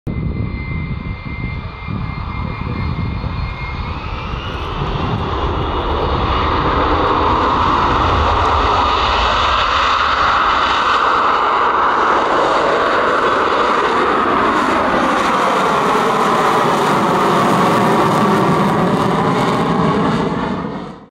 RAF F-35B strike fighter - role demo short take off
Lockheed Martin F-35B strike fighter of 207 Squadron, Royal Air Force, based at RAF Marham, demonstrating a short take off at the Royal International Air Tattoo, RAF Fairford this summer.